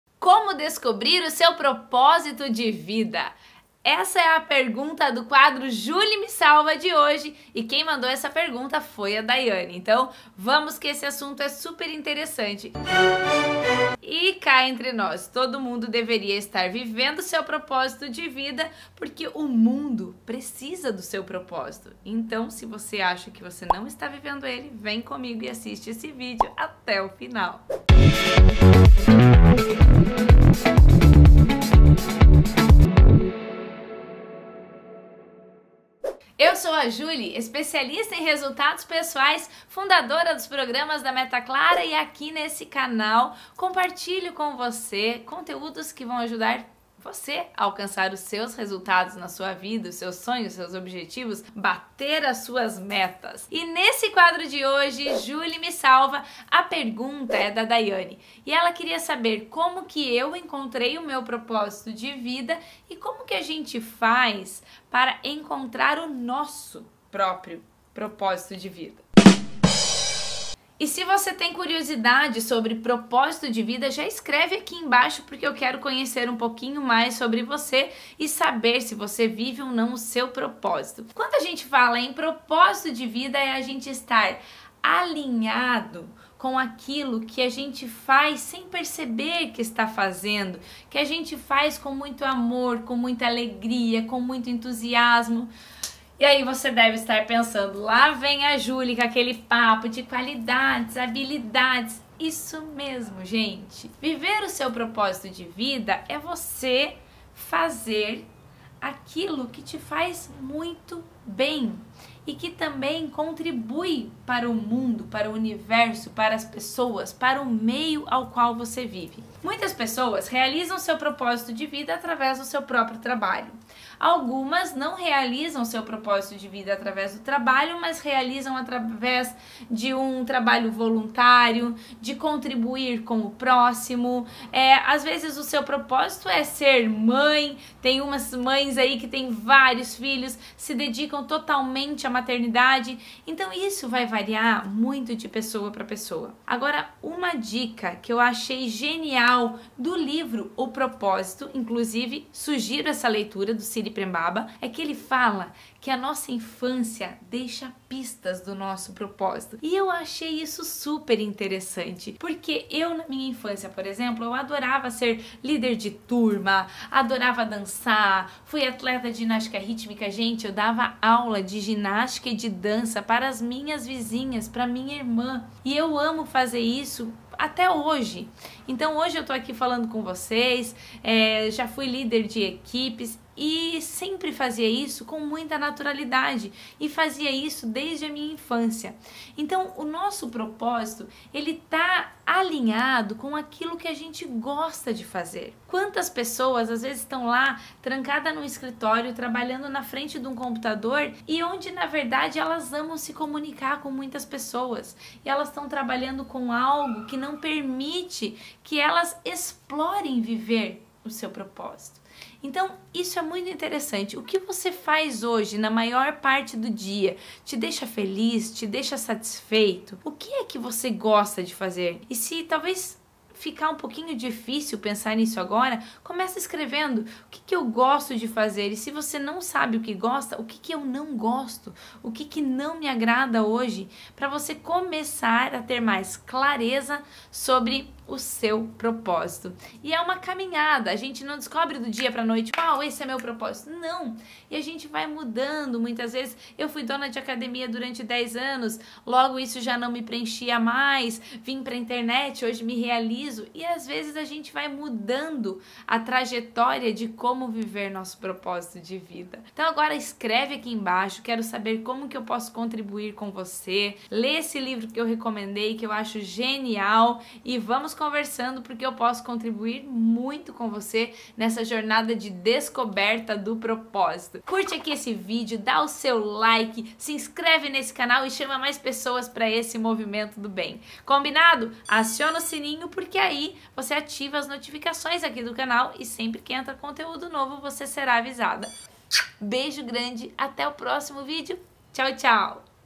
[4min] Como descobrir seu propósito de vida | JuliMeSalva EP12
Qual é o seu propósito de vida? Aquilo que dá sentido à sua vida e te realiza? Descubra qual é o seu nesse episódio de JuliMeSalva!

Beijos, Juli!